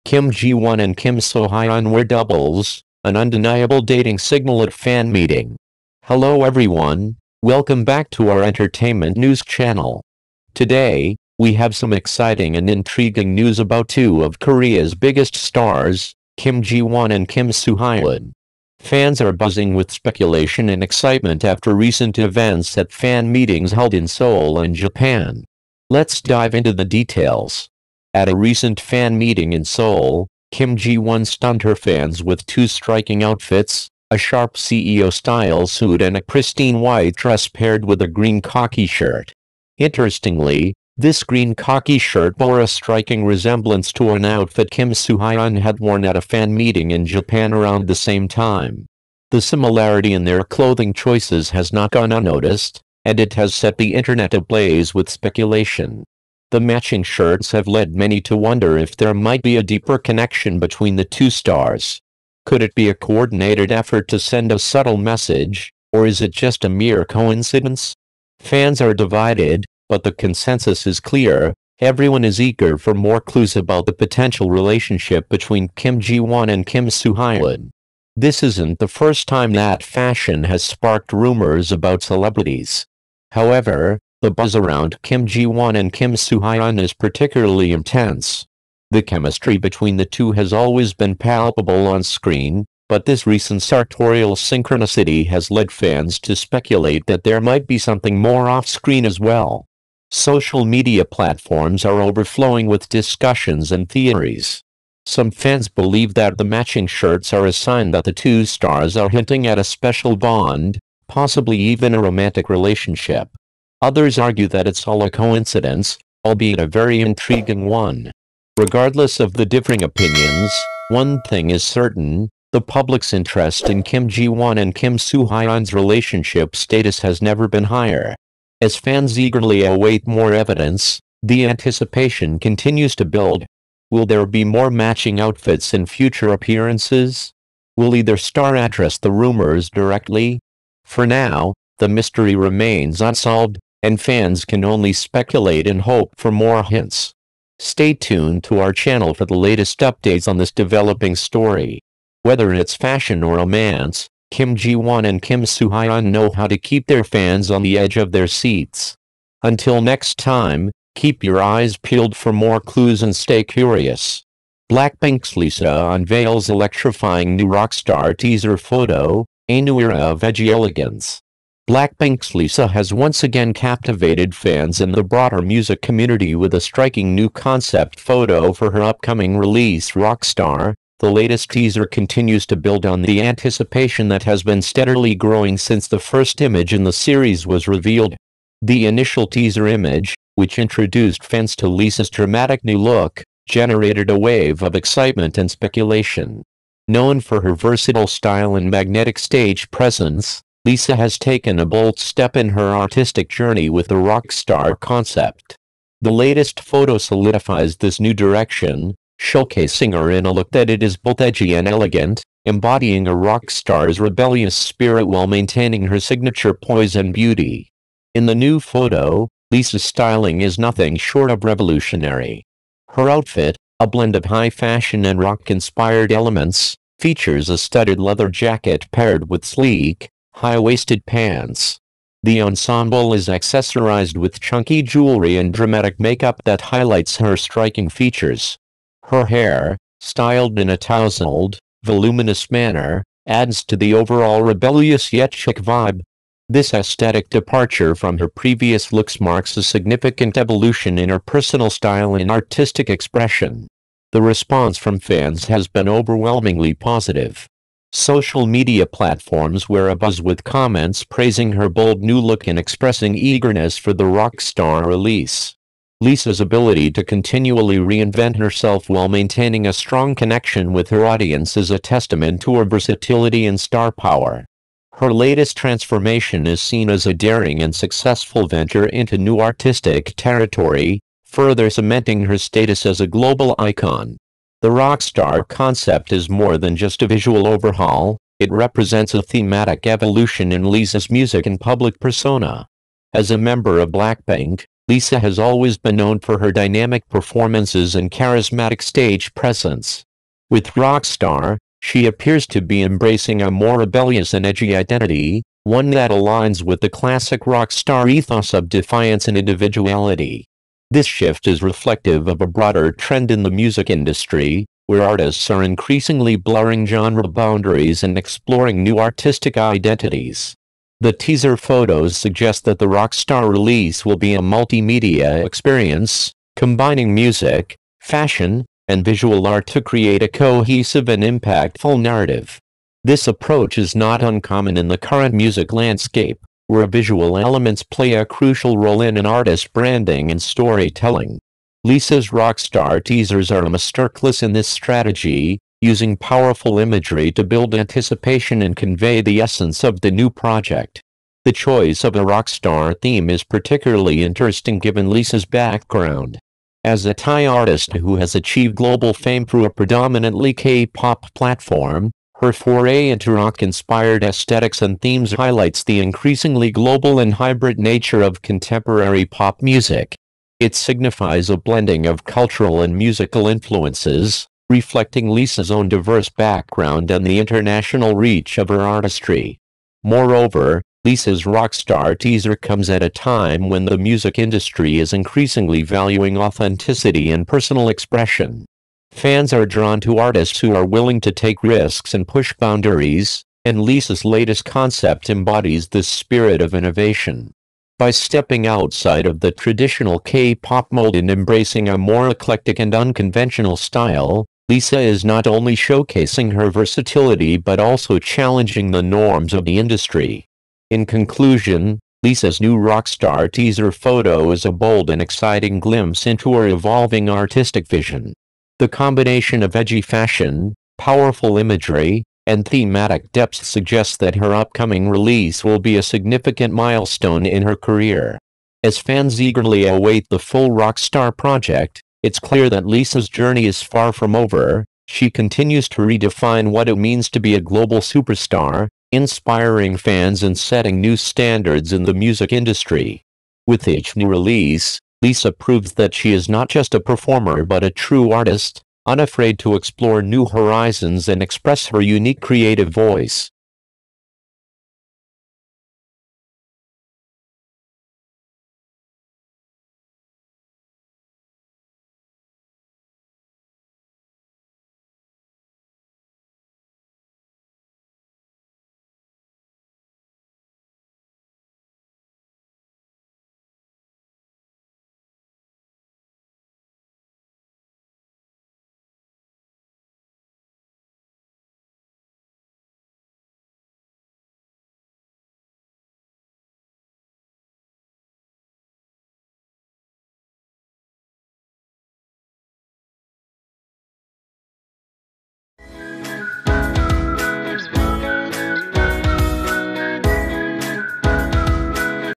Kim Ji-won and Kim so-hyun wear doubles, an undeniable dating signal at Fan M#kimjiwon
Kim Ji-won and Kim so-hyun wear doubles, an undeniable dating signal at Fan Meeting?
Important Announcement from T Gaming Channel
Best regards,
T Gaming team
Yang Yang
Xiao Zhan